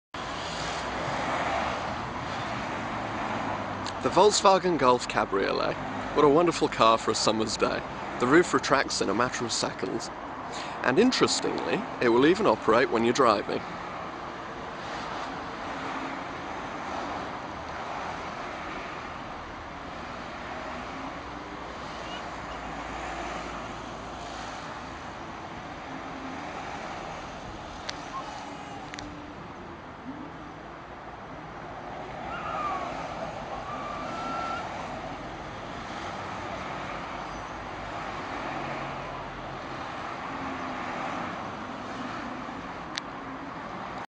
Volkswagen Golf TSi GT Cabriolet
The ultimate hairdresser's car! Watch as the Golf's roof quickly retracts in 9.5 seconds. If you watch and listen carefully you might see my baby daughter moving in the back and giving her opinion on the proceedings. Filmed by motoring journalist Tim Saunders.